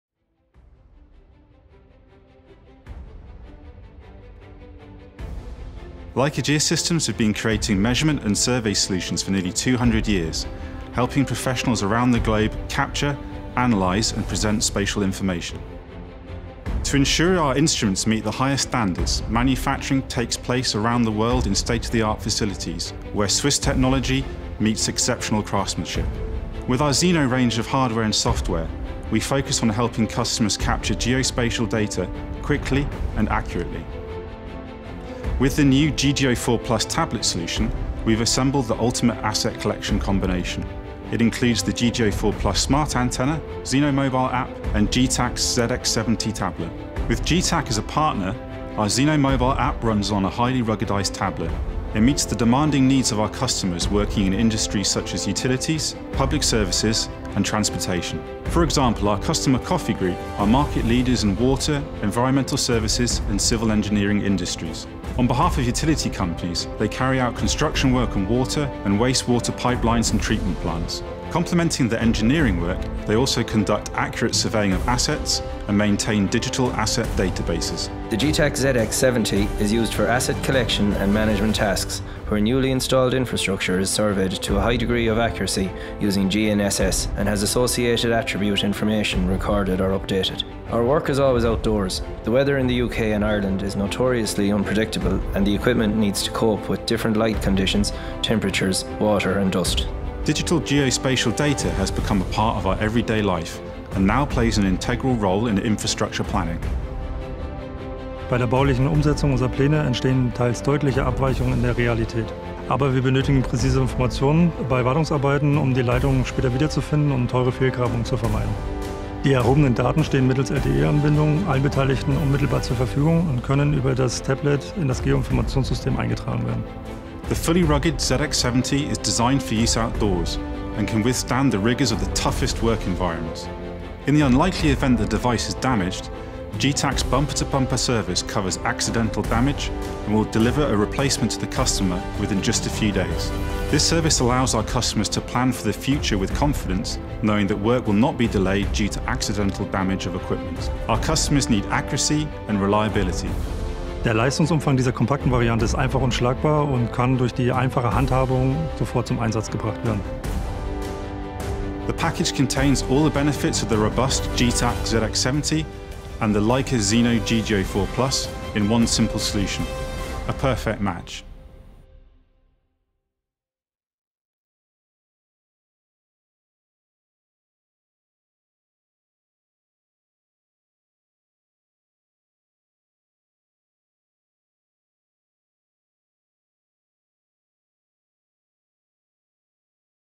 Getac Rugged Tablet & Leica Geosystems - A Perfect Match
Leica Geosystems trusts Getac’s ZX70 Tablet to power their new Zeno GGO4 Plus Solution. It brings comprehensive geospatial data collection capabilities to its customers in all terrains like Coffey Group in Ireland or the Water Industry Association in Germany.